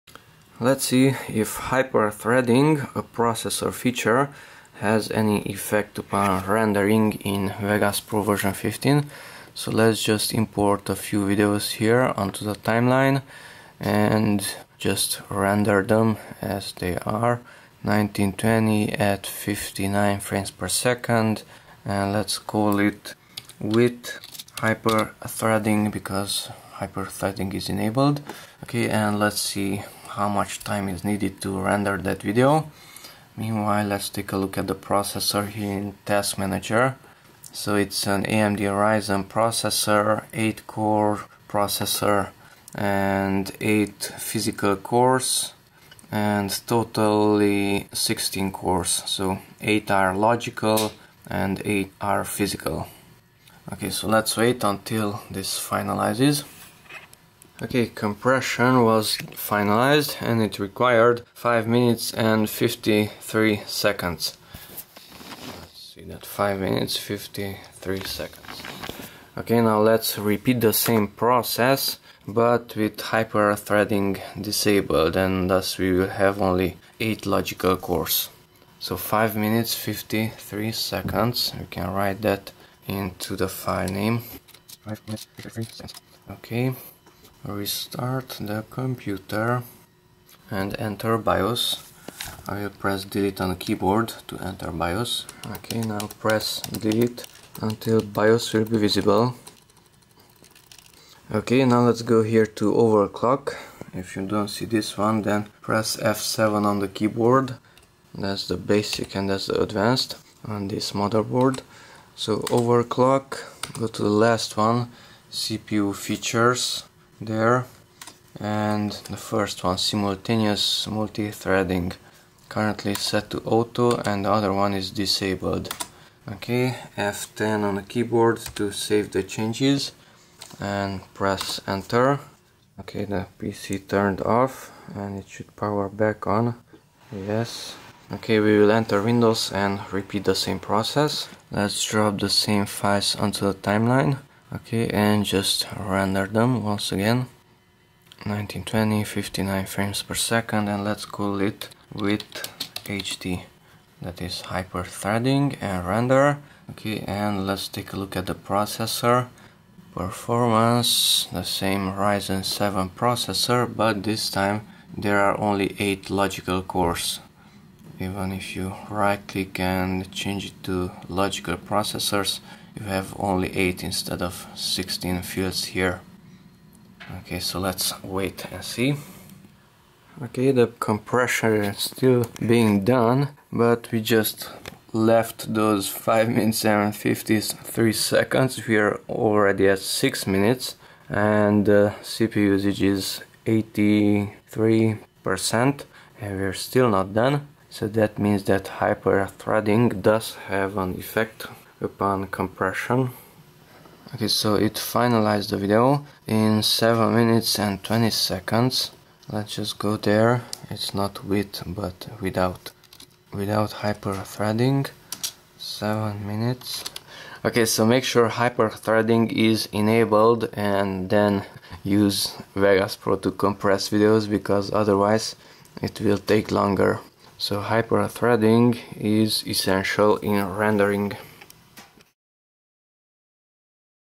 SMT (Hyper-Threading) = Faster Rendering & Lower CPU usage
Motherboard: MSI B350 PC MATE
CPU: AMD Ryzen 1700x, 3.4 GHz, 8-cores (16 when SMT enabled)

Video Editor: Vegas Pro 15

Enable SMT:
- power ON PC, press Del to enter BIOS
- go to: Advanced view (F7), OC, CPU features,
- set "Simultaneous Multi-Threading" to Auto

*Simultaneous Multi-Threading (SMT) is the equivalent of Intel's Hyper-Threading Technology